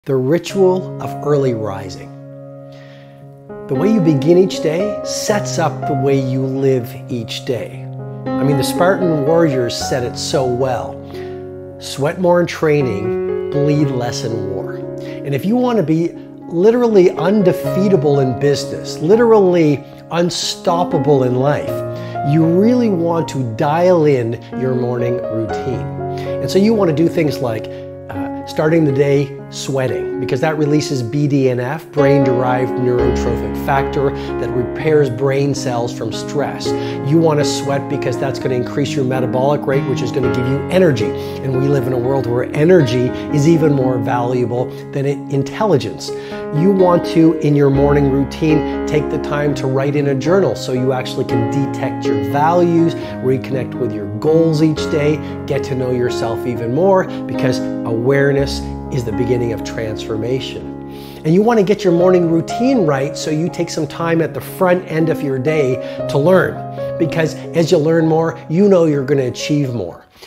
Morning Rituals (Disclosed!) | Robin Sharma | Self Improvement | Self Development
Welcome to Your Life's Keys where I serve you with everything that will inspire you to live like a champion.
Morning ritual is a powerful way to boost productivity and set intentions for the day ahead. Robin Sharma, the author of The 5AM Club, reveals wonderful self improvement, productivity, elite performance, business, the every day hero manifesto, and leadership secrets you must listen to. By establishing a consistent routine, you can create a sense of structure and stability in your life, which can lead to improved mental and emotional well-being. Whether it's meditating, exercising, or simply taking a few minutes to enjoy a cup of coffee, your morning ritual can help you start your day with a clear mind and positive attitude. By prioritizing self-care and personal growth in the morning, you can set yourself up for success in all areas of your life.